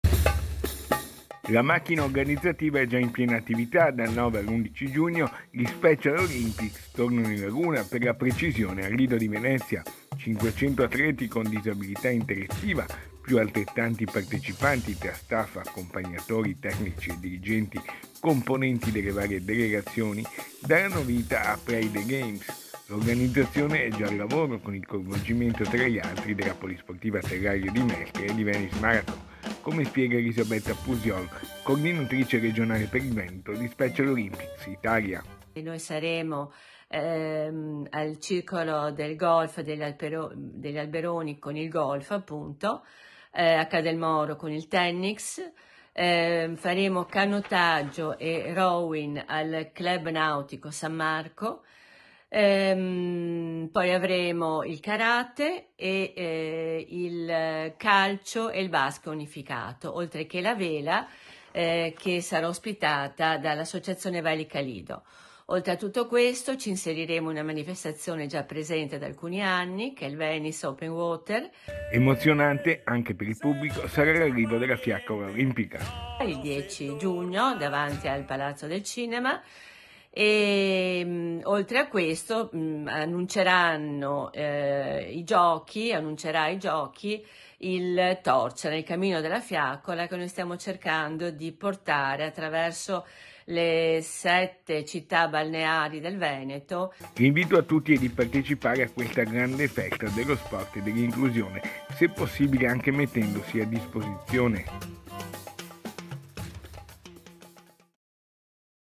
LE SPECIAL OLYMPICS TORNANO A VENEZIA IN GIUGNO | 23/03/2023
A3 NEWS Venezia 23/03/2023 - LIDO DI VENEZIA - Al Lido di Venezia dal 9 al 11 giugno arriveranno oltre 500 atleti, da tutta Italia, per partecipare alla manifestazione nazionale "Play the games" di Special Olympics Italia. La fiaccola olimpica arriverà davanti al Palazzo del cinema.  || La macchina organizzativa è già in piena attività: dal 9 all'11 giugno Gli Special Olympics tornano in laguna, per la precisione al Lido di Venezia. 500 atleti con disabilità intellettiva più altrettanti partecipanti tra staff tecnici e dirigenti, componenti delle varie delegazioni, daranno vita a "Play the Games". L'organizzazione è già al lavoro, con il coinvolgimento tra gli altri della Polisportiva Terraglio di Mestre e di Venice Marathon come spiega Elisabetta Pusiol, coordinatrice regionale per il Veneto di Special Olympics. - Intervistati ELISABETTA PUSIOL (RESPONSABILE REGIONALE VENETO SPECIAL OLYMPICS ITALIA) (Servizio di Lorenzo Mayer) - Segui Antenna Tre anche sul digitale terrestre! Visita il sito